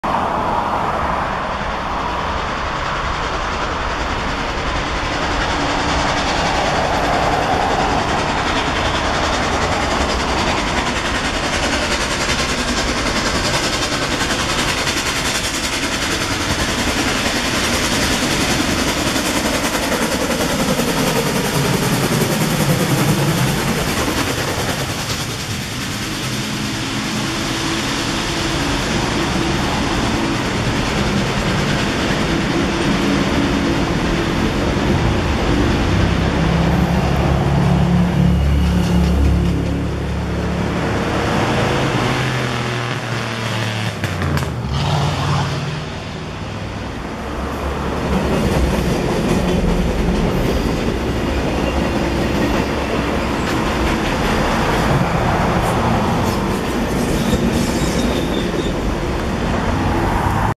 The 611 Iron Horse rolls through Linden, Virginia.
The N&W class J 611 steam locomotive graces the old Southern Railway as she powers through Linden, Virginia! 6.7.2015
filmed by SMG:D
for GlacierTV
A Dudley Production